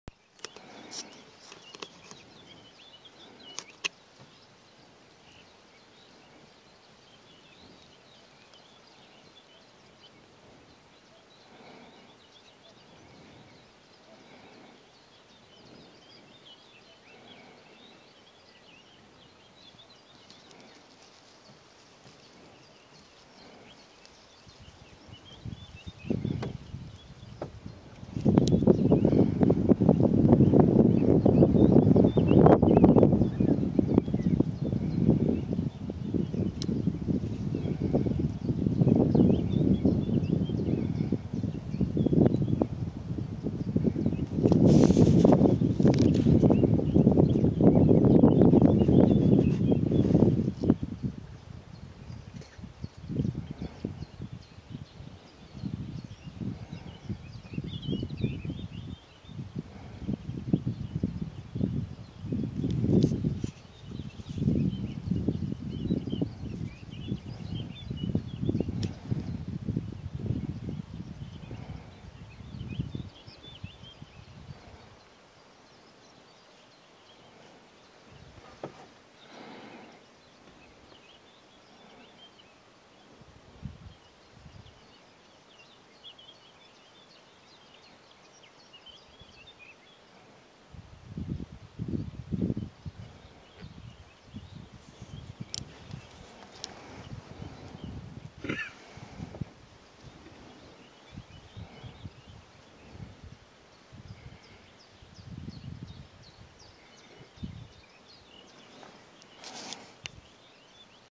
Gams im Revier Eggeralpe, Steiermark
Scharlwild am Karboden vom Revier Eggeralpe